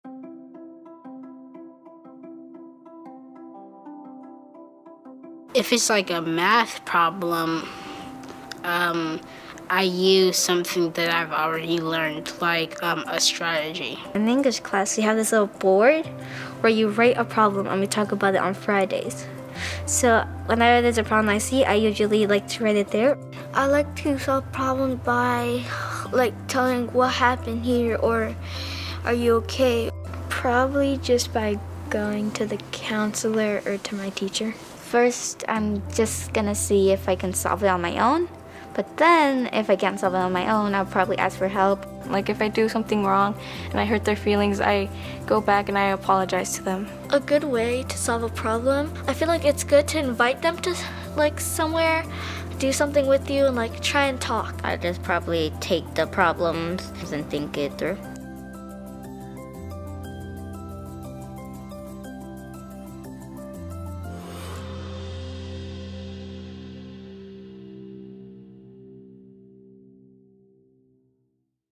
Celebrating Students: How do you solve problems?
Students work hard every day to learn new skills. To celebrate the day-to-day wins and growth that students experience on their education journey, we visited Madrona Elementary School in Highline Public Schools to hear from young people about what they are learning, how they solve problems, how they describe themselves, and more. Check out what these students have to say in our new videos! Thank you to Communities in Schools Federal Way-Highline, Madrona Elementary, and Highline Public Schools for your partnership!